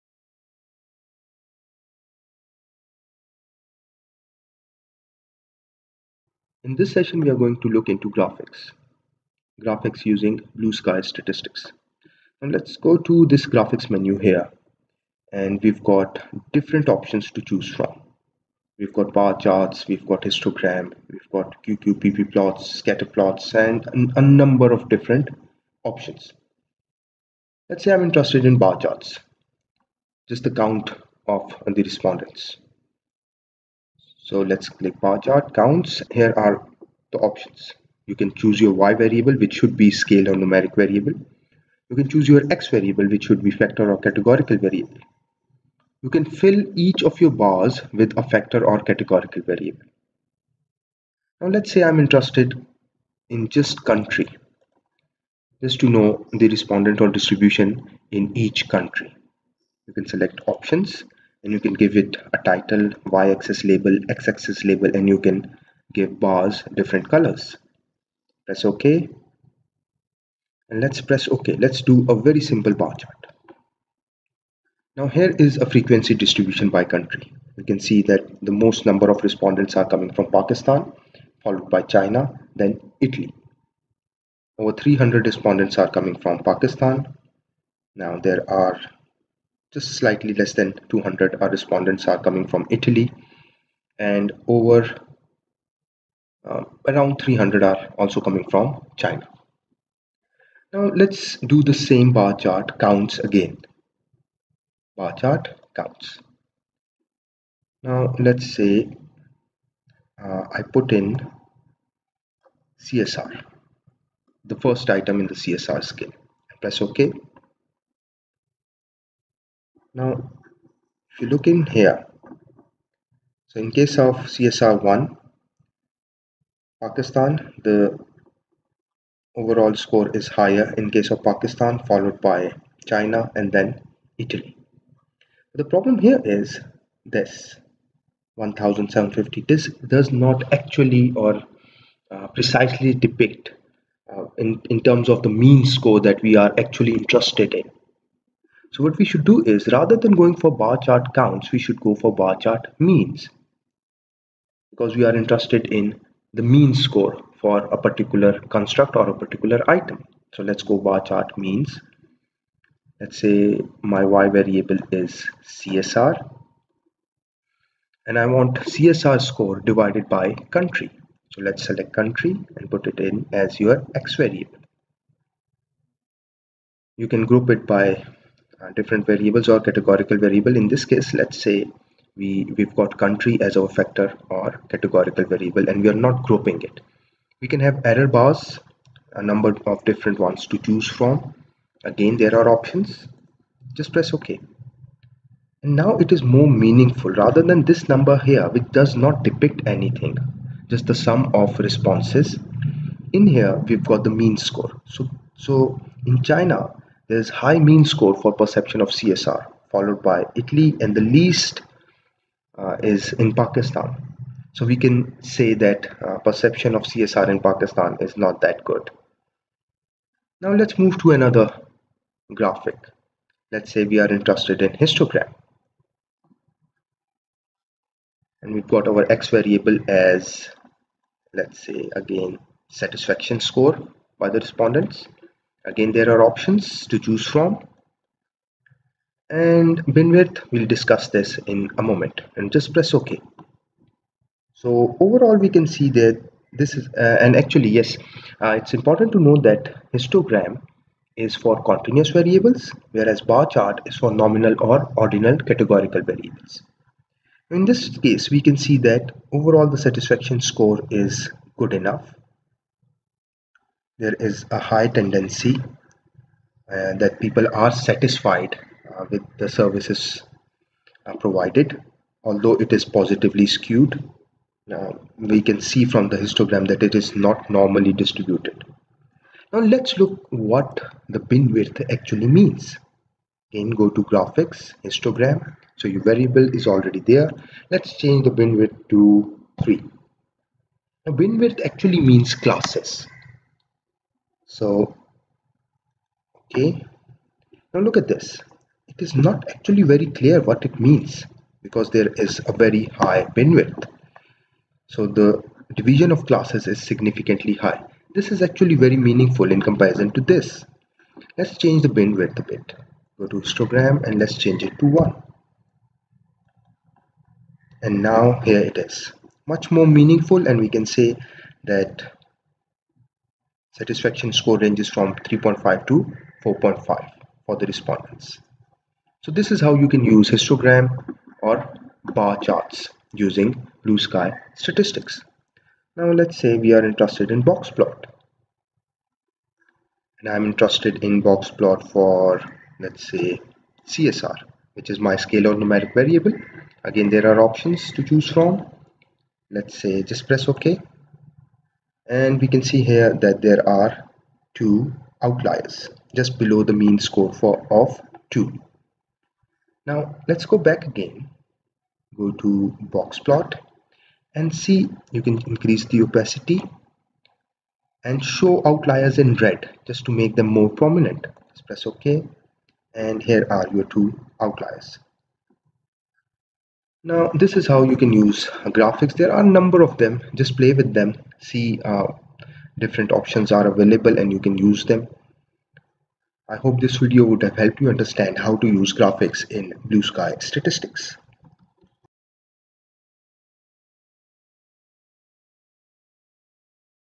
Graphics in BlueSky Statistics - Bar Chart, Histogram, BoxPlot
The video guides on how to use Graphics like Bar Chart, Histogram, and Box Plot using BlueSky Statistics